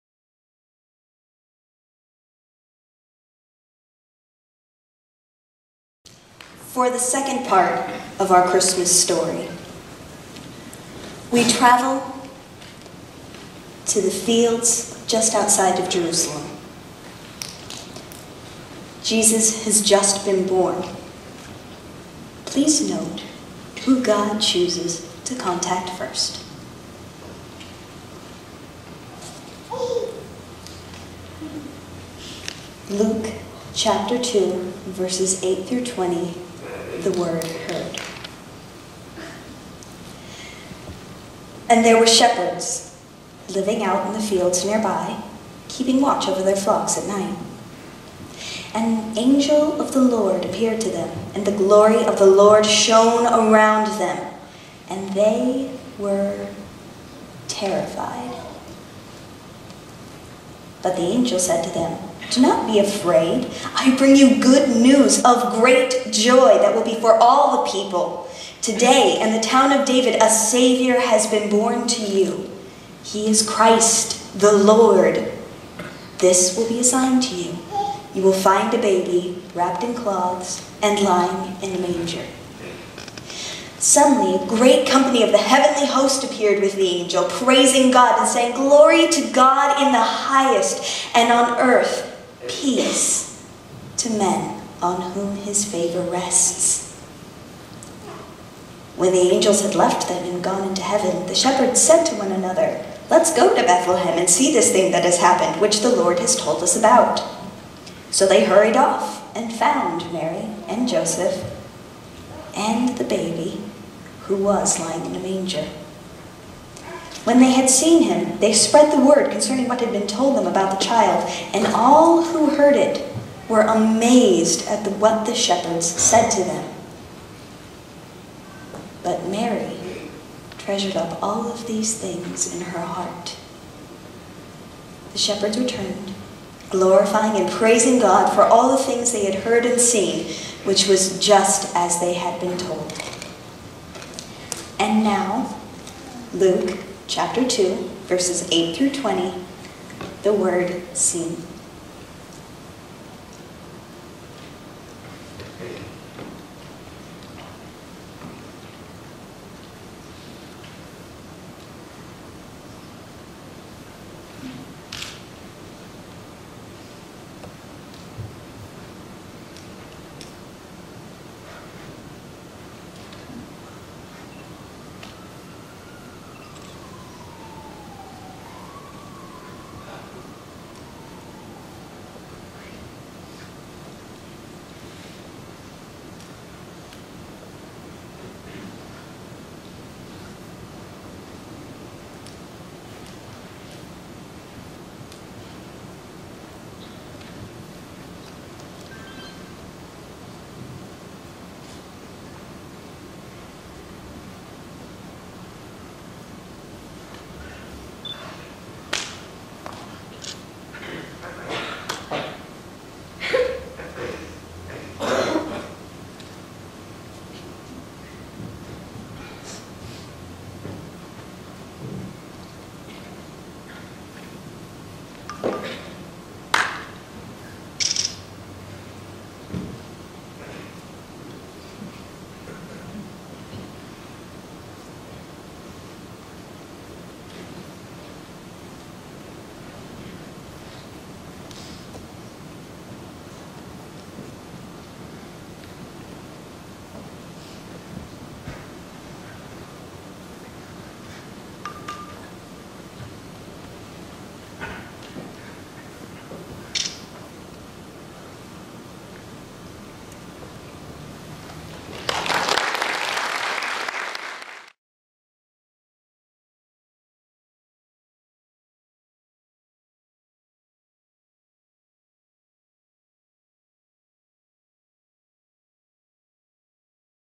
The Word Heard The Word Seen pt 2
2 churches, Deaf & hearing share a Christmas program. Here, the sheperd's story is first read, then told ASL storytelling.